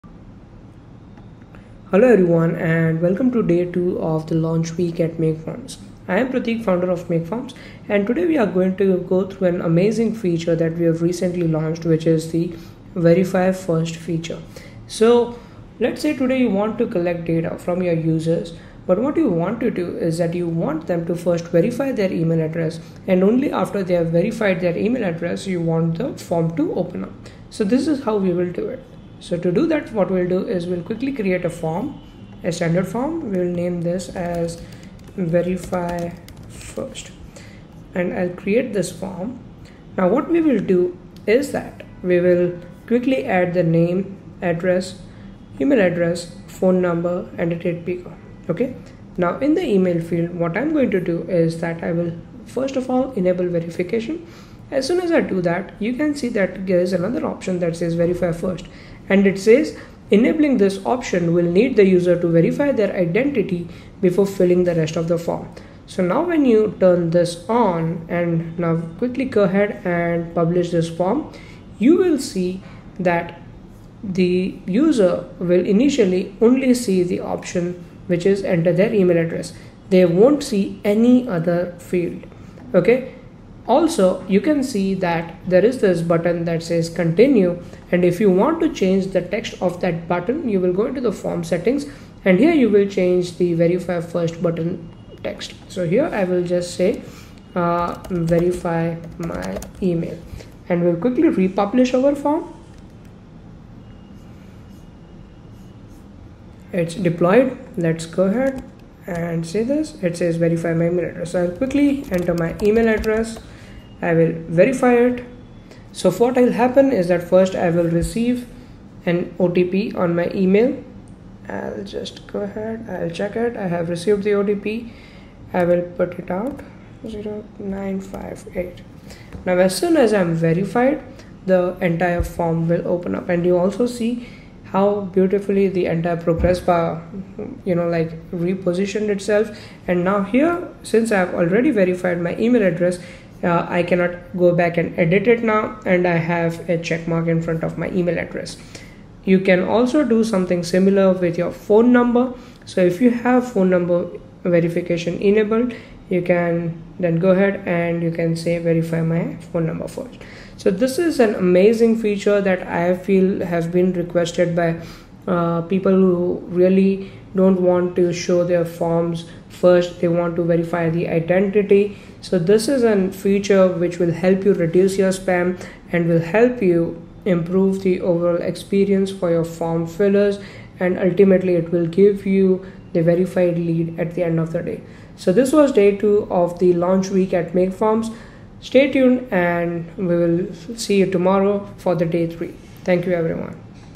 MakeForms Launch Week: Day 2 - Introducing the Verify First Feature!
Welcome to Day 2 of MakeForms Launch Week! In this video, Pratik, the founder of MakeForms, introduces a new and powerful feature: Verify First.

What is the Verify First Feature?
This added layer of verification helps reduce spam and guarantees that only verified leads are received.

Creating the Form: Start by creating a standard form and naming it appropriately.
Enabling Verification: In the email field, enable verification and turn on the "Verify First" option. This ensures users must verify their email before accessing the rest of the form.
Customization: The text on the verify button can be customized to match specific needs. For example, change the button text to "Verify My Email."
Verification Process: Users will receive an OTP on their email. Once verified, the full form becomes accessible, and they can complete it. The email field will show a checkmark indicating successful verification.

Benefits:
Reduce Spam: By verifying user identity first, spam entries are significantly reduced.
Improve User Experience: The verification process enhances the overall experience for form fillers.
Get Verified Leads: Ensure that every form submission is from a verified user, leading to more reliable data and leads.

Additional Features:
Phone Number Verification: Similar to email verification, phone number verification can also be enabled to ensure even more robust identity checks.

Thank you for joining Day 2 of MakeForms Launch Week.